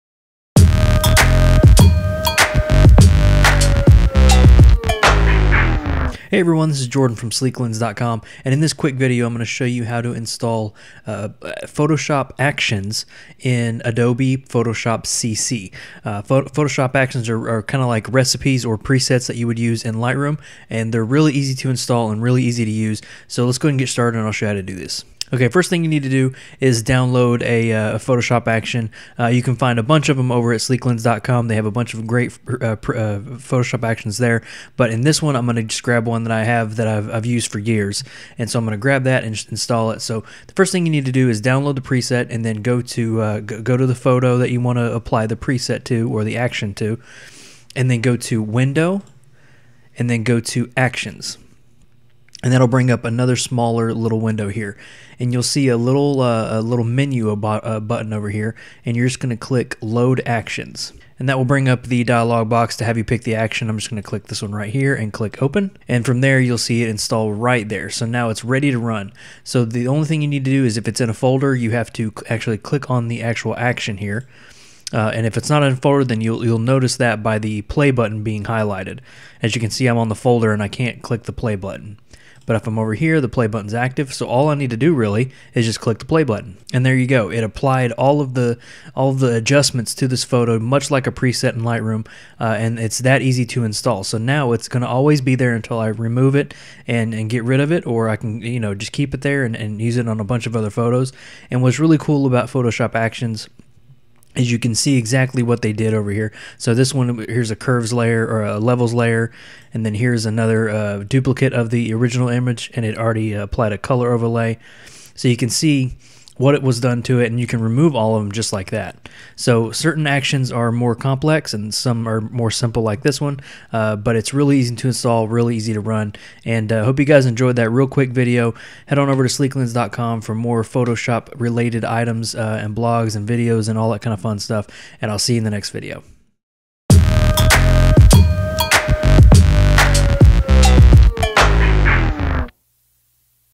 How to Install Actions in Adobe Photoshop CC
In this guide, we are going to cover the entire procedure for installing our Sleeklens Actions in Adobe Photoshop CC.

7 different Photoshop Bundles of Photoshop Actions which will help us to craft our way to masterpieces in a fast, easy manner with a professional quality outcome:

• Compatible with Photoshop 4-6 and Adobe Creative Cloud
• An easy-to-follow guide on how to install these Photoshop Actions